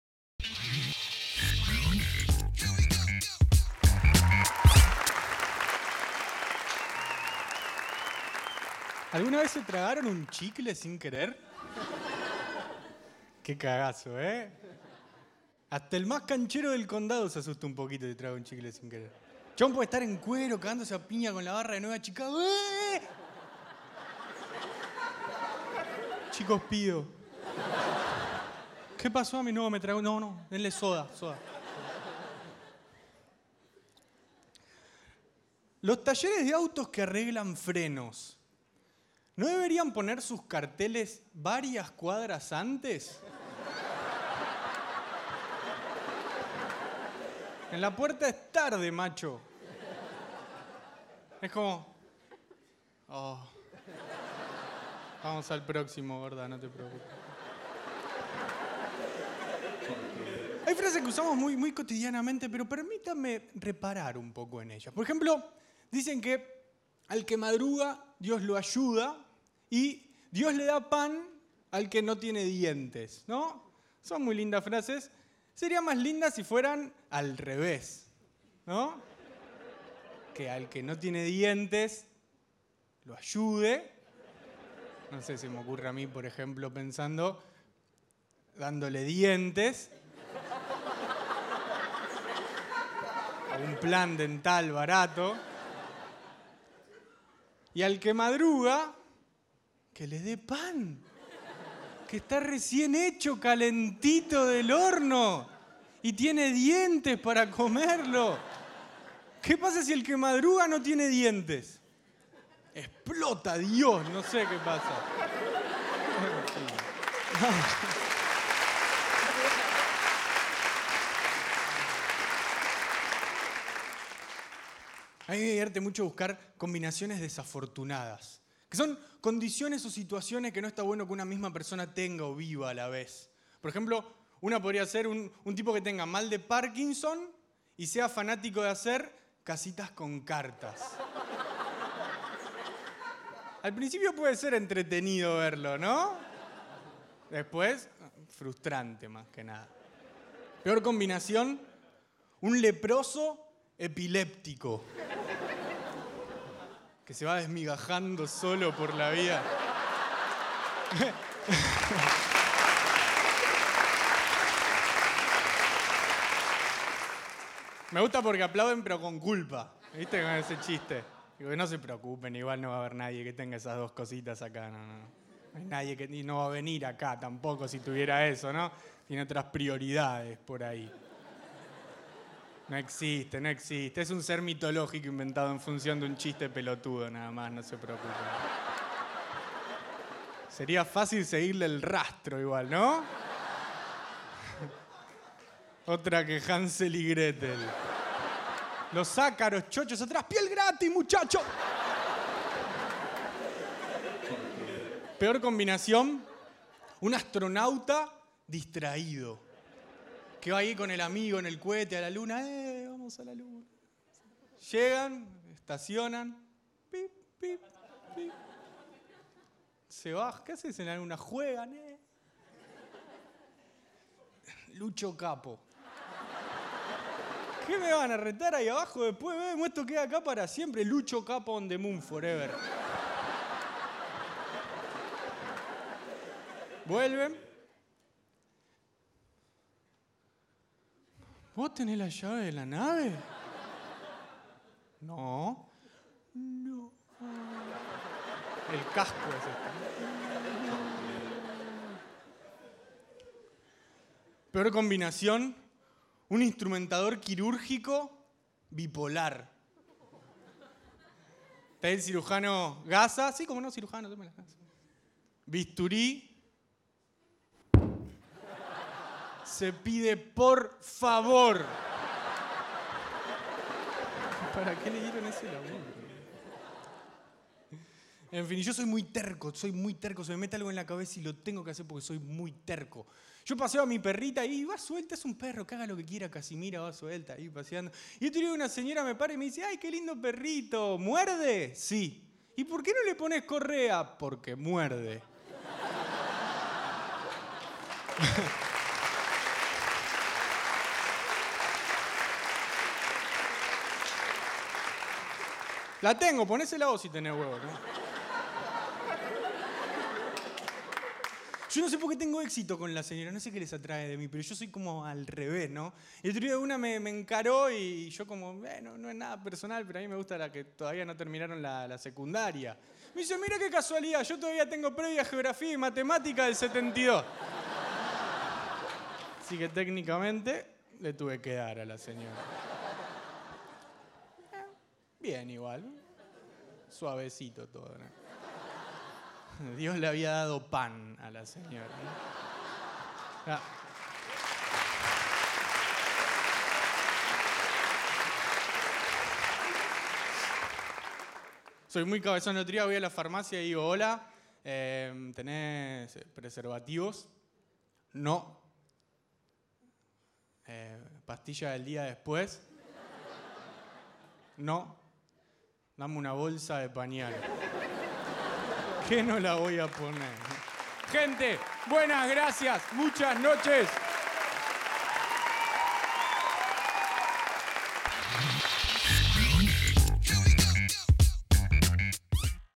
Luciano Mellera 2014
Querés verme en vivo? Mirá acá

Luciano Mellera, Comediante Argentino de Stand Up. Se formó con Fernando Sanjiao y Martín Pugliese.